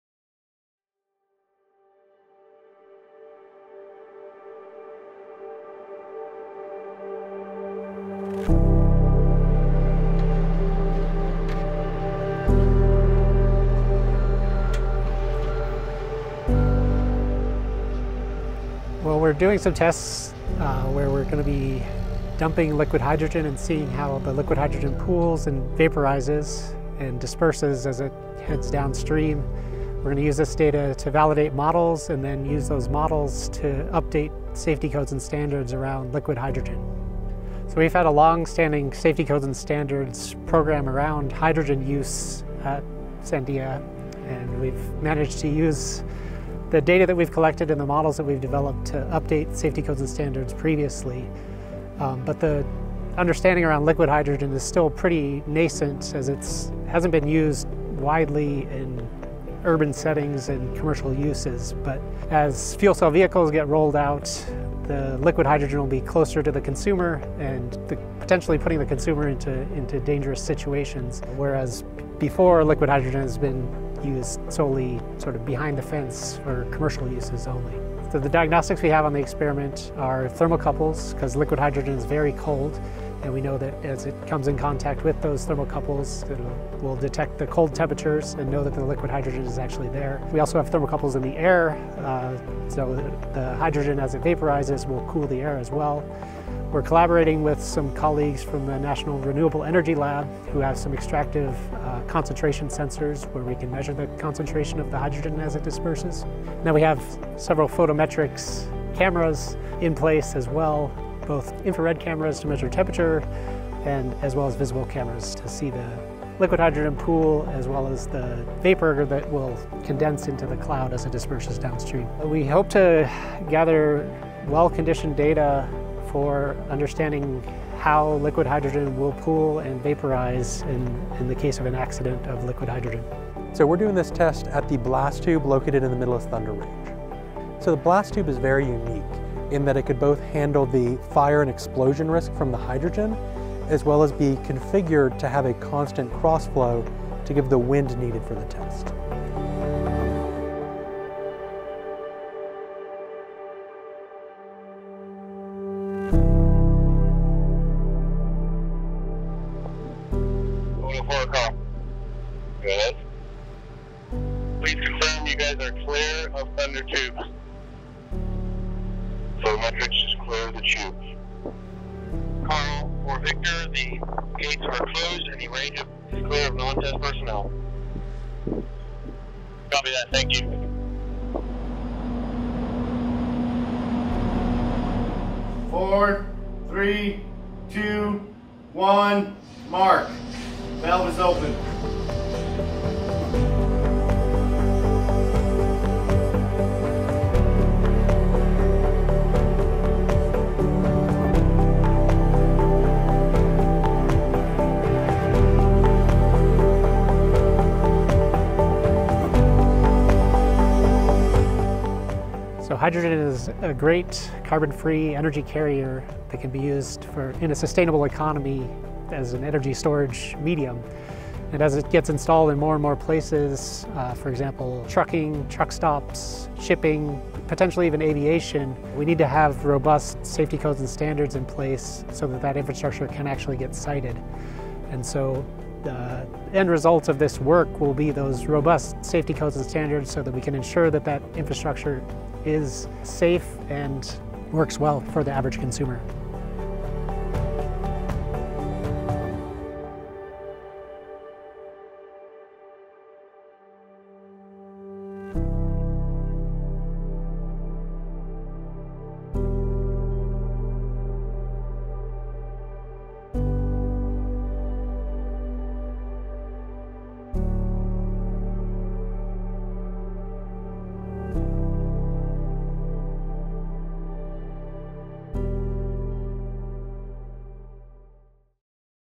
Sandia Liquid Hydrogen Pooling Experiments: Validating Models for Safety Codes and Standards
Sandia contributes to the science of advanced hydrogen and fuel cell technologies by providing a deep, quantitative understanding and scientific basis for materials interaction with hydrogen and safety. A series of successful tests at Sandia National Laboratories collected data to validate models used for safety codes and standards, supporting efforts to assess the safety of hydrogen infrastructure. The work was supported by the US DOE Office of Energy Efficiency & Renewable Energy (EERE) Hydrogen and Fuel Cell Technologies Office (HFTO).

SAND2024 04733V